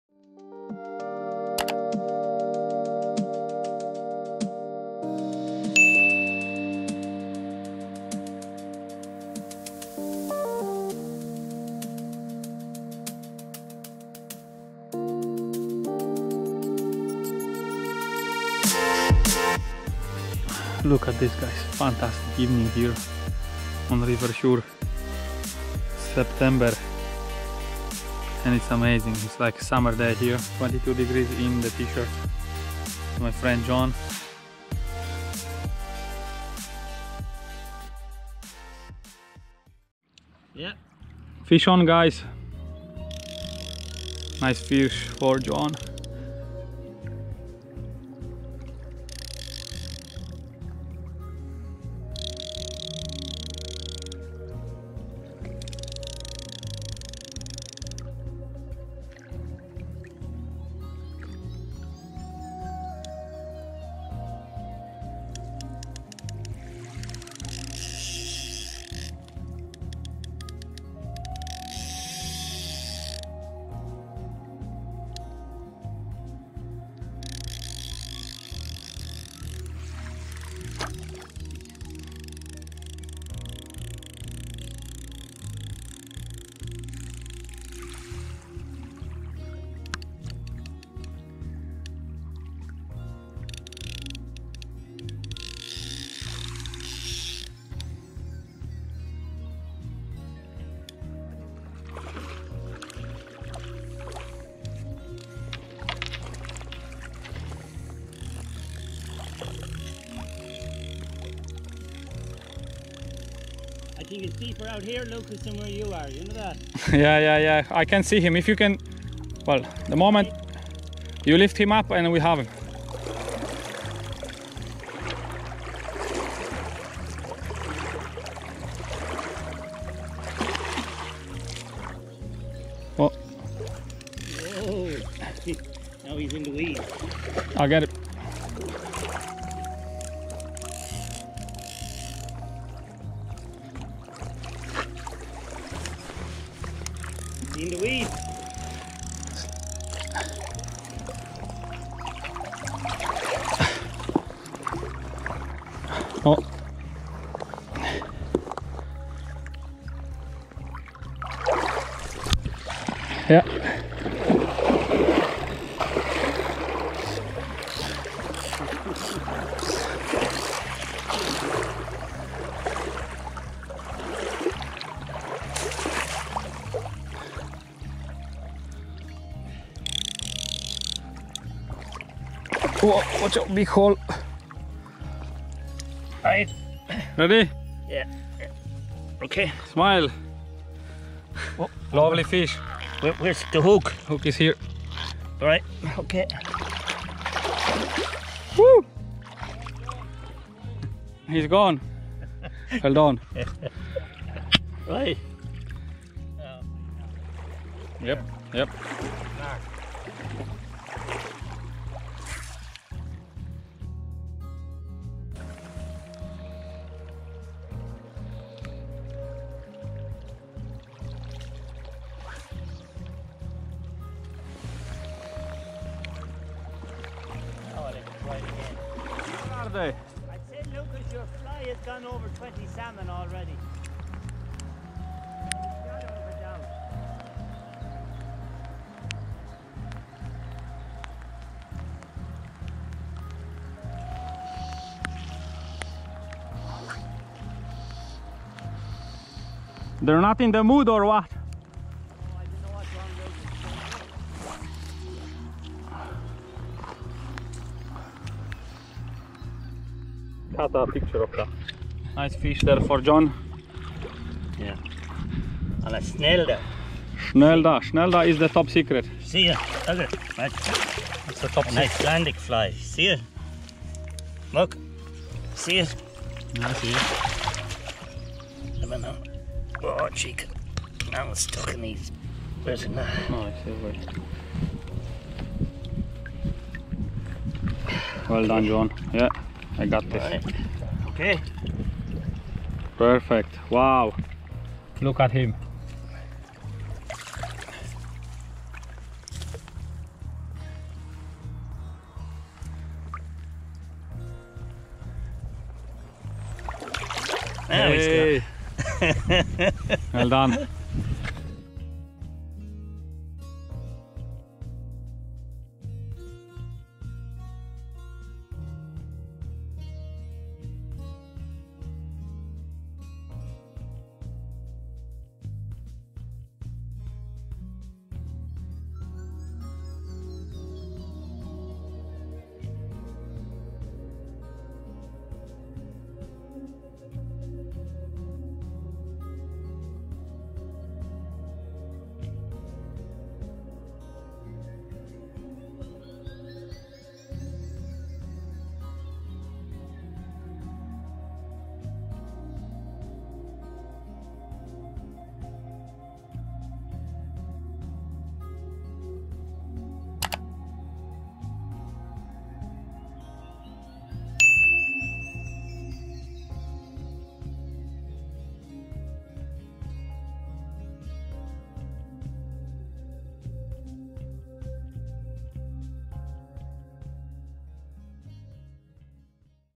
Fly fishing for salmon with John Lalor - Ireland
Edits from a few fishing trips we had this year with John,
Focusing on Casting more than catching...
Often used only one fly rod..
Thanks to John for sharing his knowledge with me,
John Lalor author of the book: ‘The River Suir’ A Blue Riband River for Trout & Salmon Fishing.
Published by: Mayfly Lodge Publishing, Cahir, Co. Tipperary
See you again soon!